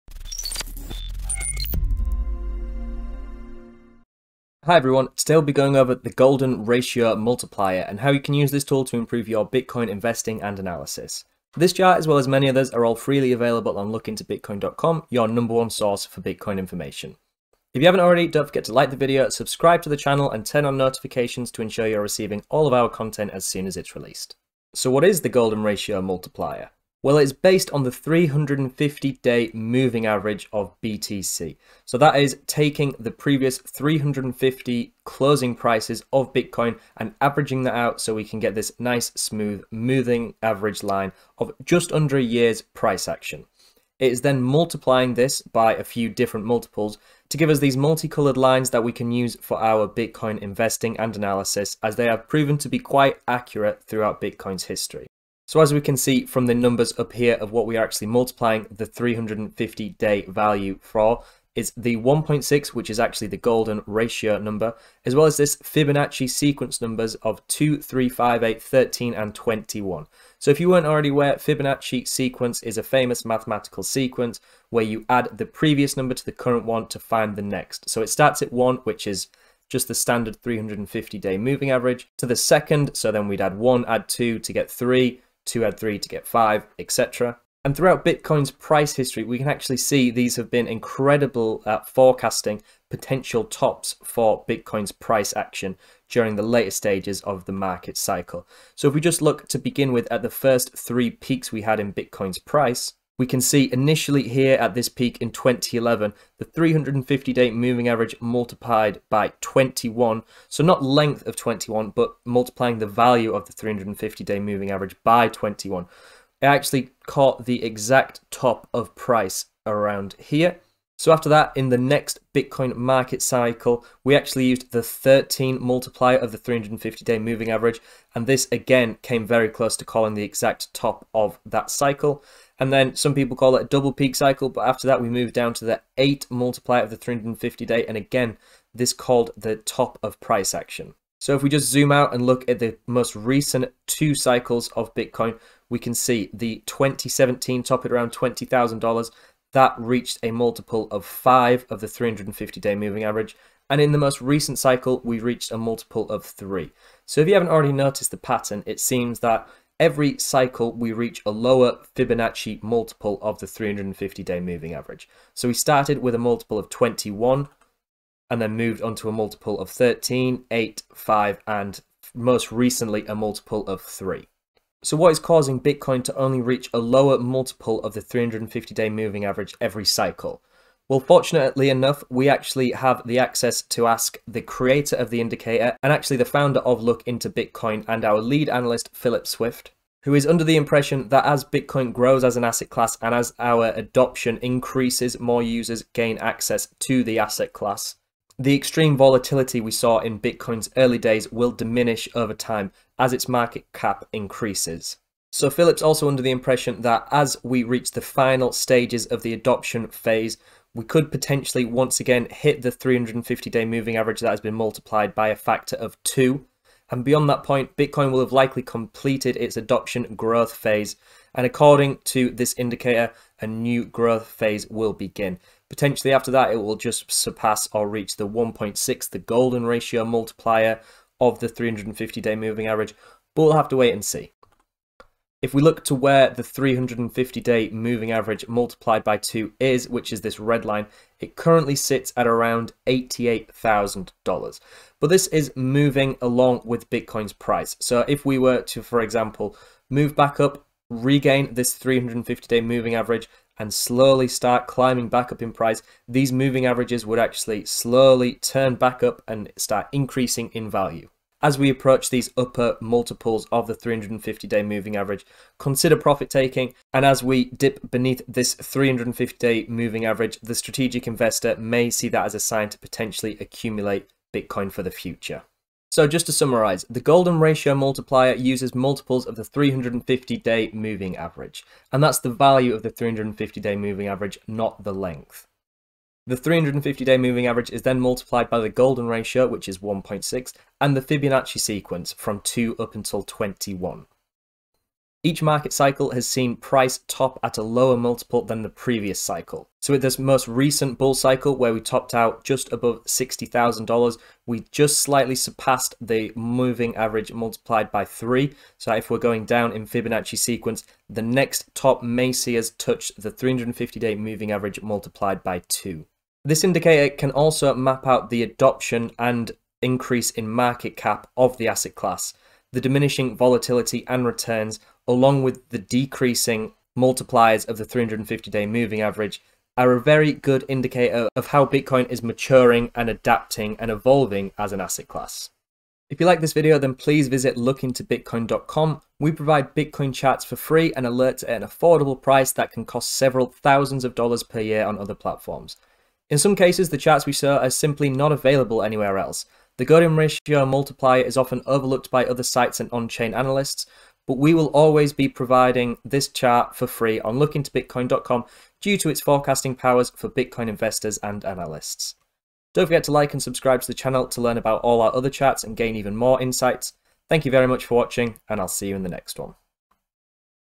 The Golden Ratio Multiplier
The Golden Ratio Multiplier is a Bitcoin chart that explores Bitcoin's adoption curve and market cycles to understand how price may behave on medium to long term time frames.

To do this it uses multiples of the 350 day moving average (350DMA) of Bitcoin's price to identify areas of potential resistance to price movements.

The multiples reference the Golden Ratio (1.6) and the Fibonacci sequence (0, 1, 1, 2, 3, 5, 8, 13, 21). These are important mathematical numbers.

These specific multiplications of the 350DMA have been very effective over time at picking out intracycle highs for Bitcoin price and also the major market cycle highs.